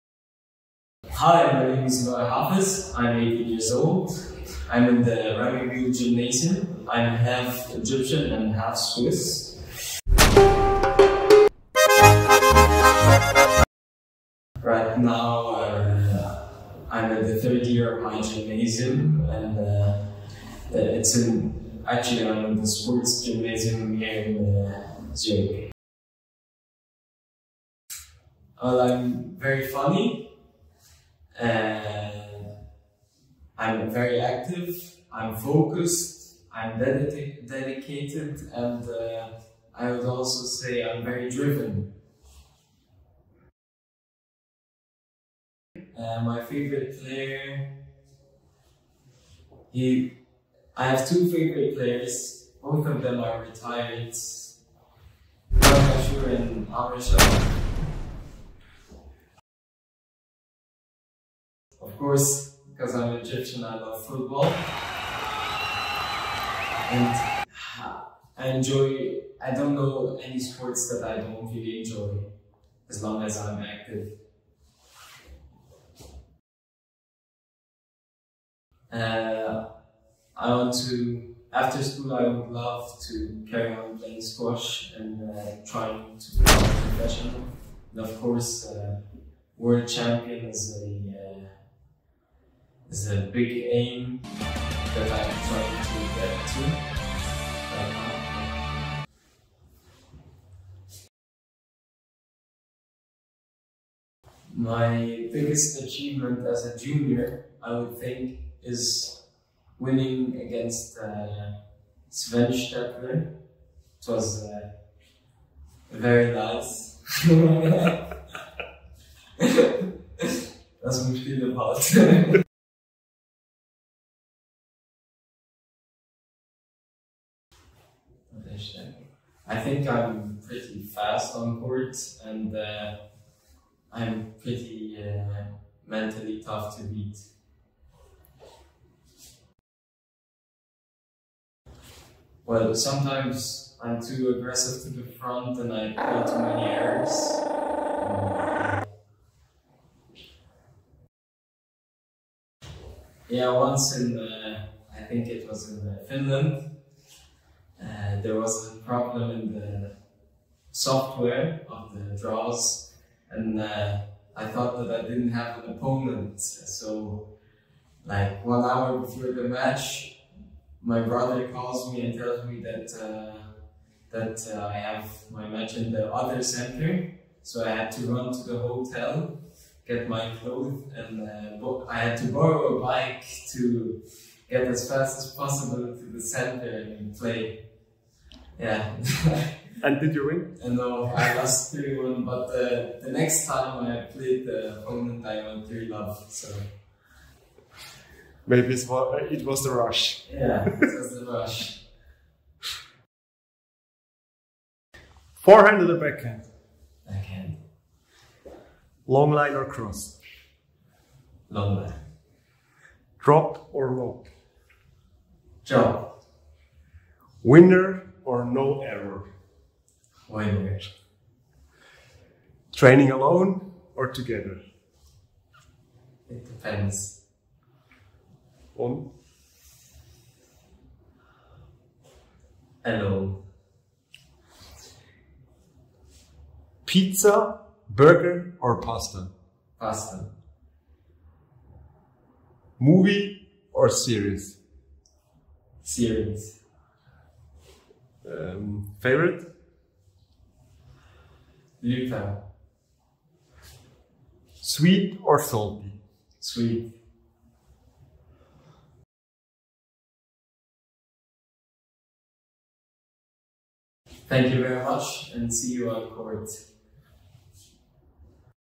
Interview mit Louai Hafez GC NLA Team
Louai Hafez: Swiss/Egypt Squash-NextGen Player from GC NLA Team im Interview.